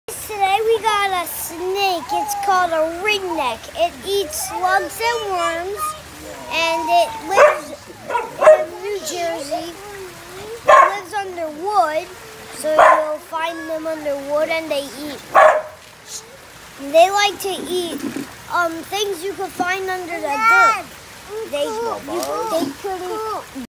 Ring neck snake
Found a baby ring neck snake! Don’t worry I Released him to where I found him! Grayish-brown with orange ring and belly.